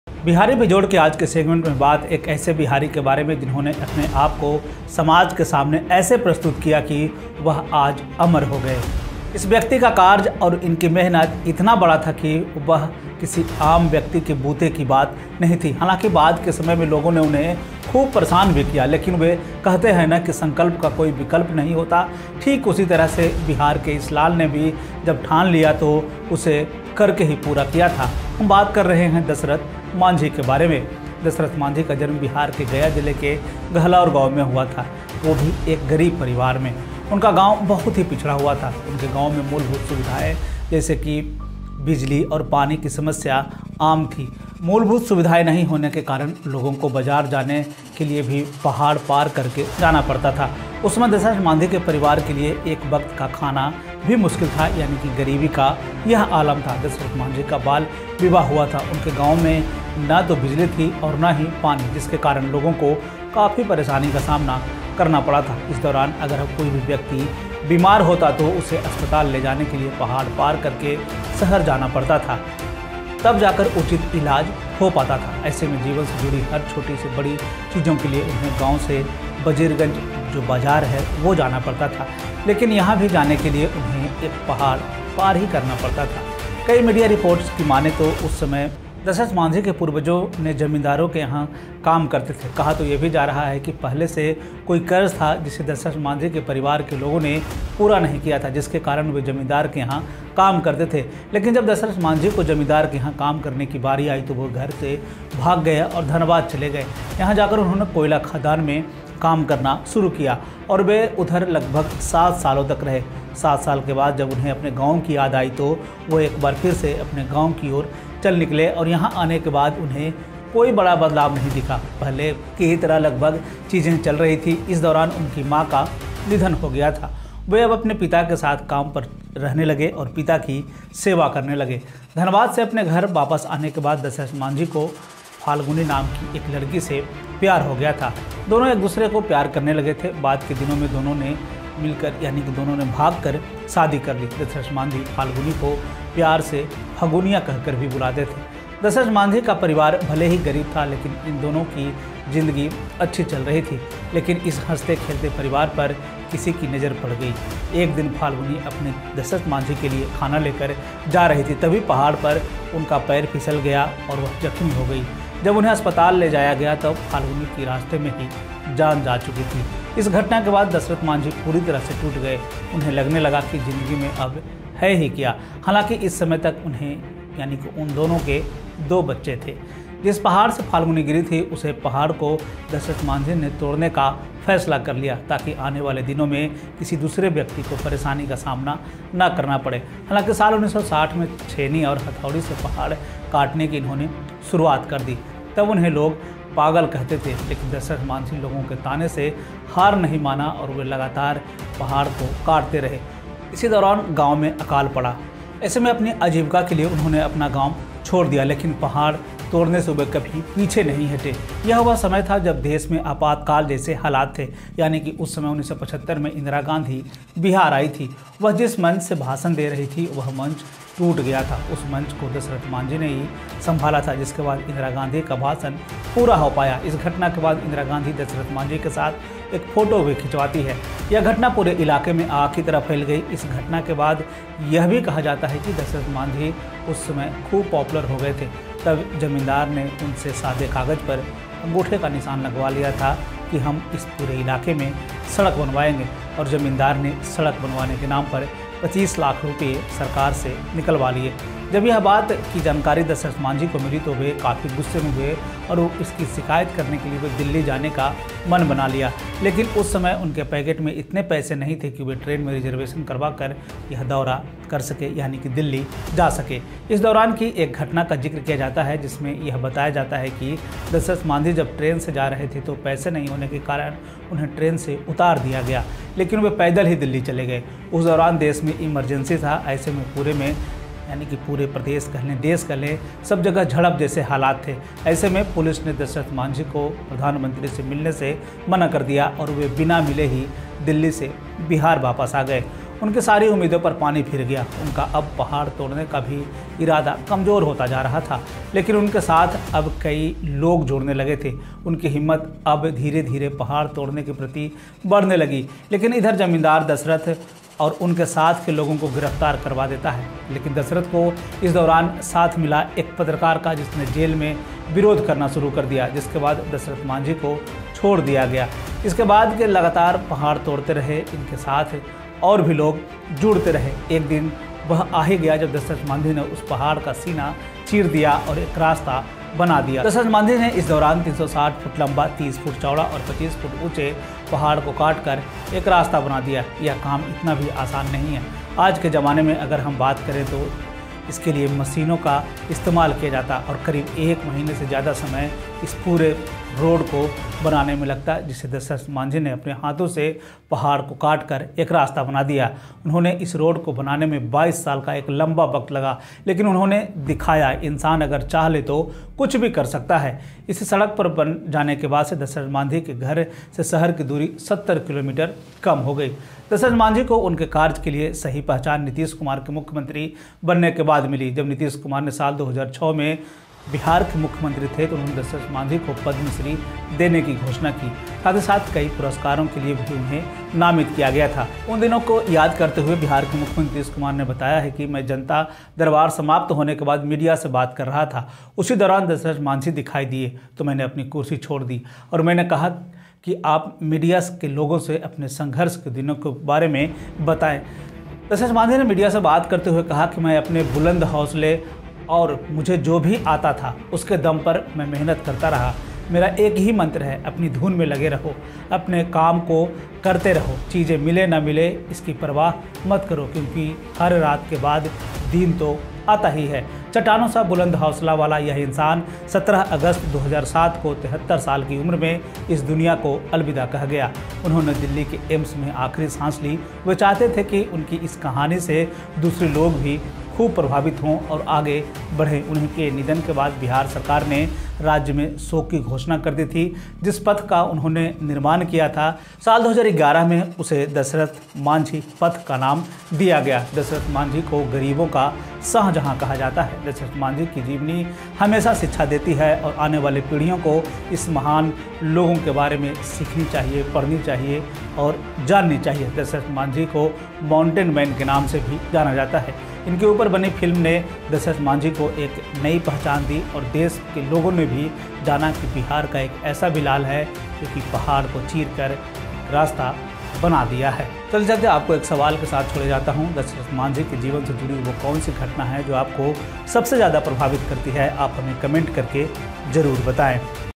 जिसके प्यार को लोगों ने समझा मजाक, उसके लिए CM ने छोड़ दी अपनी कुर्सी
Dashrath Manjhi was born in a poor family in Gehlaur village of Gaya district of Bihar. His village was very backward. There were no basic facilities in his village. Because of which people had to cross the mountain to go to the market. At that time it was difficult for Dashrath Manjhi's family to even have one meal. Dashrath Manjhi was married as a child. There was neither electricity nor water in his village, due to which people had to face a lot of trouble. During this time, if any person fell ill, he had to cross the mountain and go to the city to take him to the hospital. Only then could proper treatment be done.